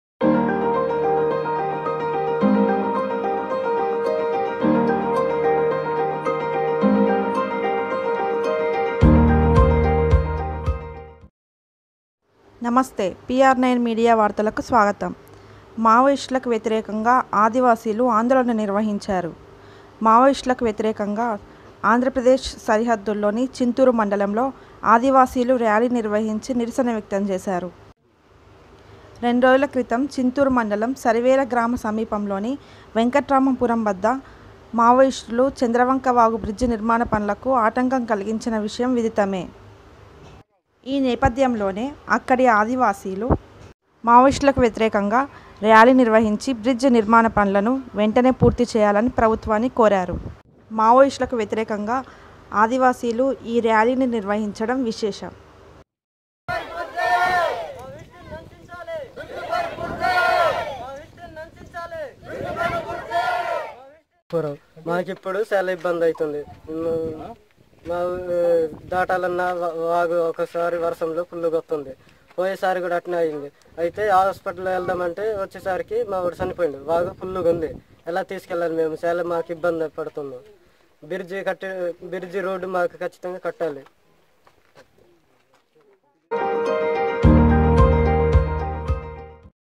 ఆంధ్రా సరిహద్దుల్లో మావోయిస్టులకు వ్యతిరేకంగా ఆదివాసీలా ఆందోళన..!
ఆంధ్ర ప్రదేశ్: (08/06/20)

మావోయిస్టులకు వ్యతిరేకంగా ఆదివాసీల ఆందోళన..!

మావోయిస్టులకు వ్యతిరేకంగా ఆంధ్రా సరిహద్దుల్లోని తూర్పు గోదావరి జిల్లా చింతూరు మండలంలో ఆదివాసీలు ఆందోళన నిర్వహించారు. రెండు రోజుల క్రితం చింతూరు మండలం సరివేల గ్రామ సమీపంలోని వెంకట్రామపురం వద్ద మవోయిస్టులు చంద్రవంక వాగుపై జరుగుతున్న బ్రిడ్జి నిర్మాణ పనులకు ఆటంకం కలిగించిన విషయం విధితమే. ఈ బ్రిడ్జి నిర్మాణ పనులకు మావోయిస్టులు అడ్డుపడాన్ని అక్కడి ఆదివాసీలు వ్యతిరేకిస్తూ సోమవారం ర్యాలీ నిర్వహించి, నిరసన వ్యక్తం చేశారు. బ్రిడ్జి నిర్మాణ పనులను వెంటనే పూర్తి చేయాలని వారు ప్రభుత్వాన్ని కోరారు.